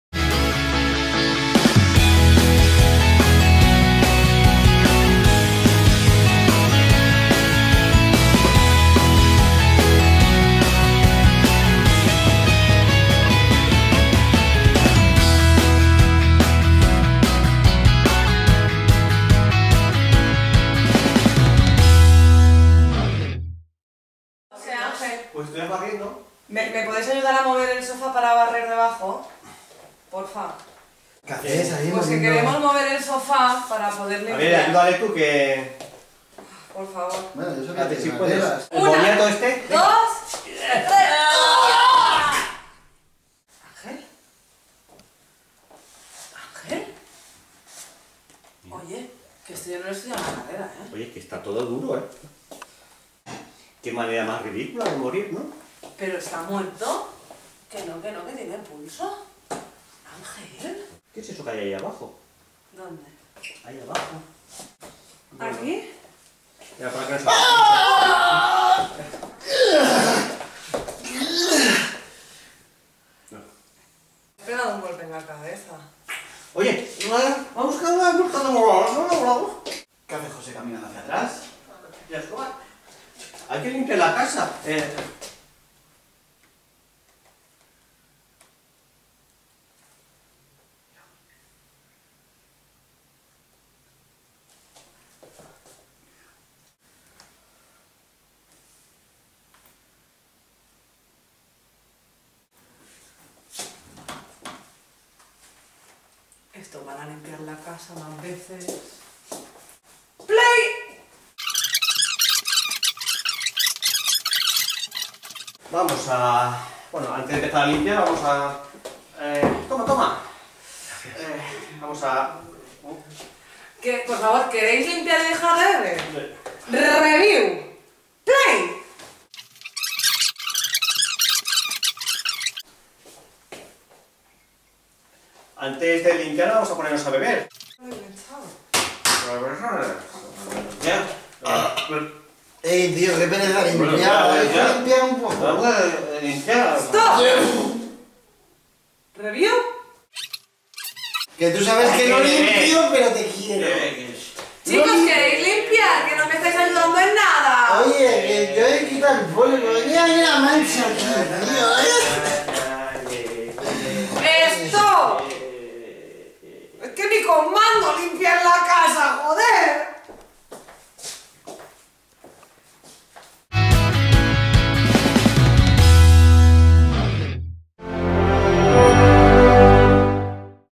EL SOFÁ DE IMPRESENTABLES CAPITULO 5 EL MANDO
Engel, Neus y Jose hacen limpieza. Encuentran un mando muy especial.